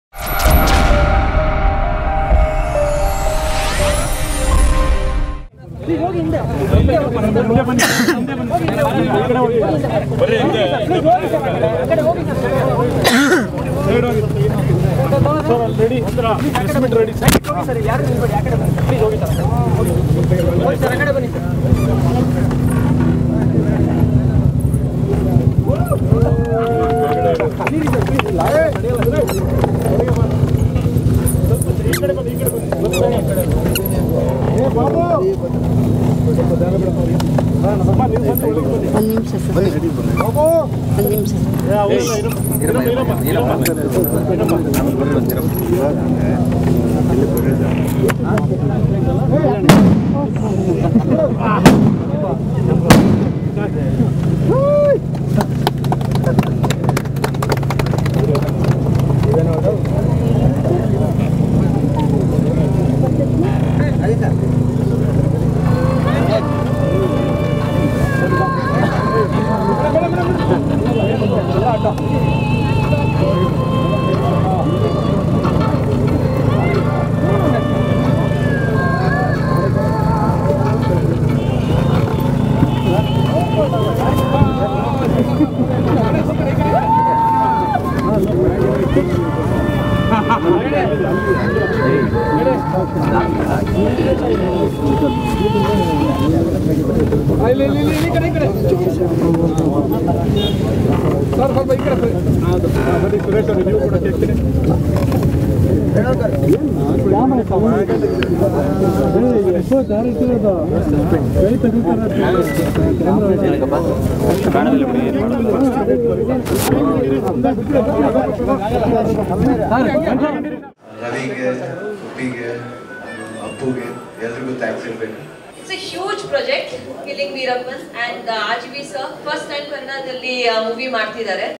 Exclusive: Golden Star Ganesh's 38th Birthday Cake Cutting, 2015
Golden Star Ganesh recently celebrated his 38th birthday with his new film ' Pataki' team. A must watch.